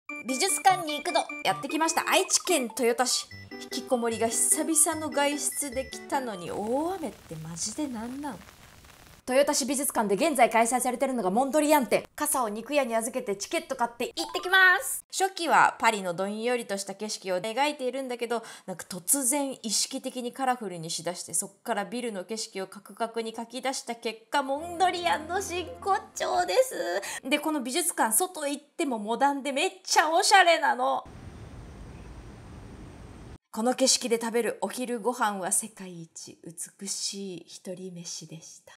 美術館いくど！豊田市美術館「モンドリアン展」バウハウス
ピート・モンドリアン
生誕150年記念 モンドリアン展 純粋な絵画をもとめて
ラジオ最新回（毎週木曜）
【通常版】

毎回一人の画家にフォーカスしてお届けするネットラジオ番組『そんない美術の時間』
「アートを身近に」